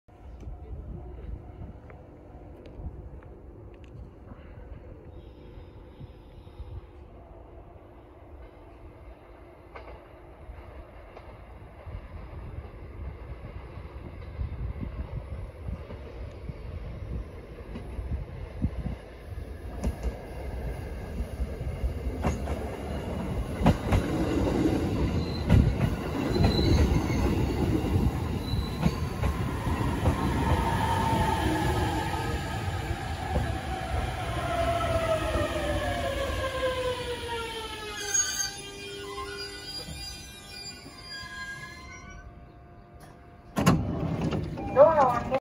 E531系 JR水戸線 大和駅 到着
JR水戸線の大和駅にE531系車両が到着しました。
大和駅は茨城県桜川市にある単線区間で無人駅です。